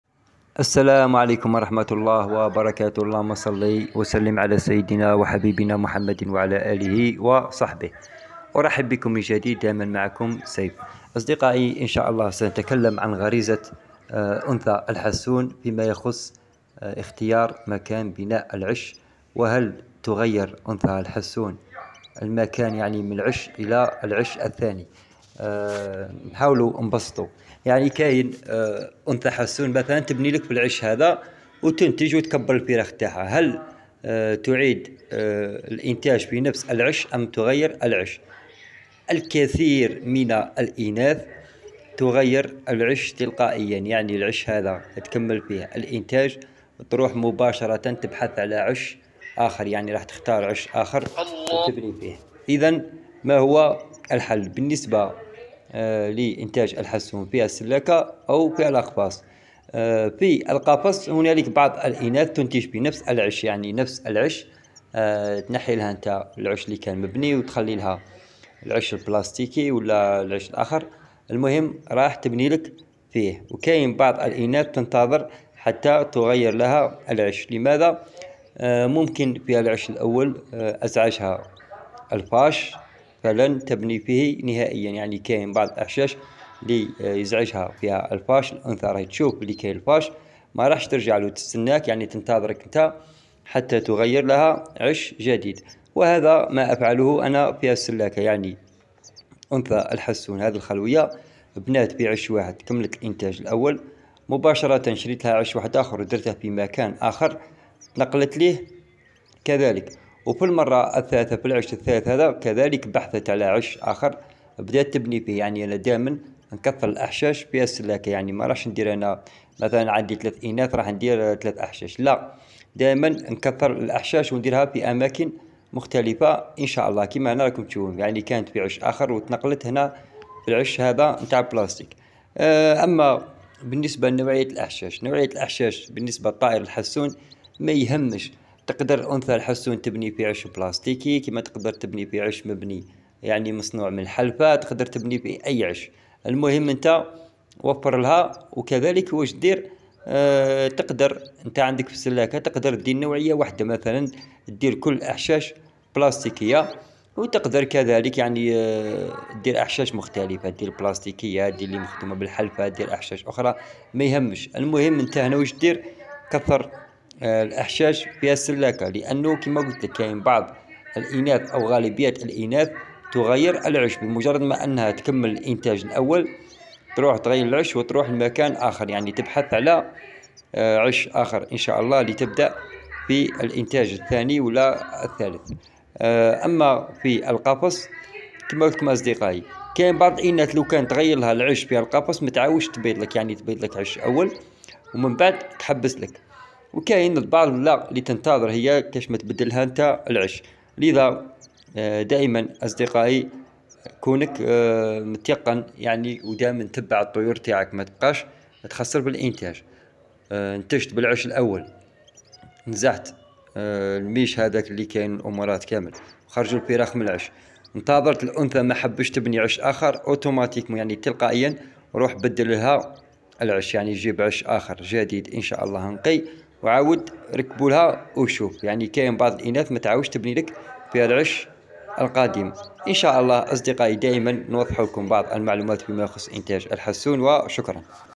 هل نغير عش أنثى الحسون بعد البطن الأول أو ندع القديم وكيف
هل أغير عش أنثى الحسون بعد الإنتاج الأول